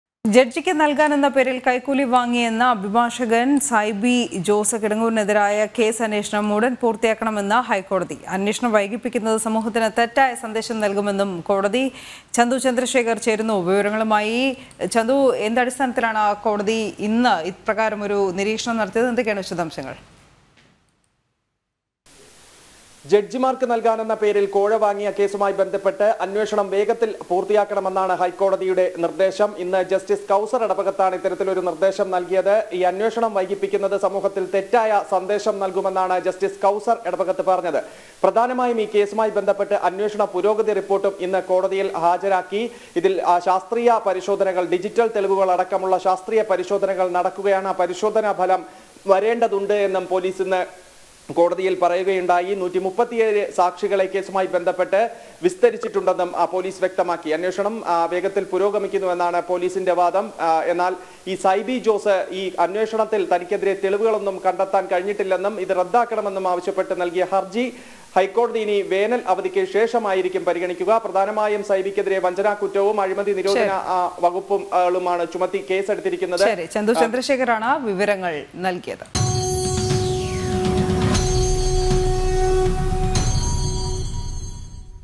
അഭിഭാഷകൻ സൈബി ജോസ് കിടങ്ങൂരിനെതിരായ കേസ് അന്വേഷണം ഉടൻ പൂർത്തിയാക്കണമെന്ന് ഹൈക്കോടതി
ജഡ്ജിക്ക് നൽകാനെന്ന പേരിൽ കൈക്കൂലി വാങ്ങിയെന്ന അഭിഭാഷകൻ സൈബി ജോസ് കിടങ്ങൂരിനെതിരായ കേസ് അന്വേഷണം ഉടൻ പൂർത്തിയാക്കണമെന്ന് ഹൈക്കോടതി